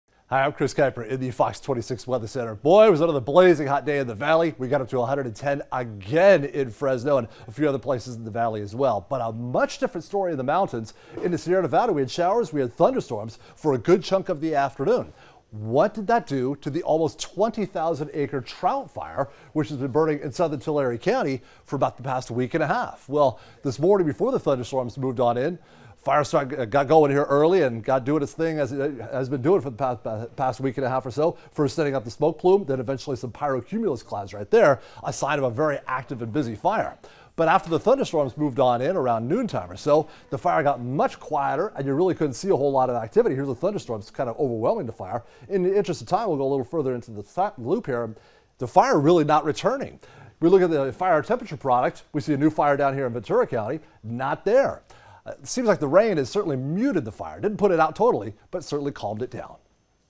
Meteorologist Kris Kuyper gives an update on the Trout Fire burning in Tulare County
What happened to the 20,000 acre Trout Fire, burning in Tulare County?
Thunderstorms that swept through the southern Sierra Nevada today didn't put a fire that large out... but meteorologist Kris Kuyper shows us here how they really calmed it down.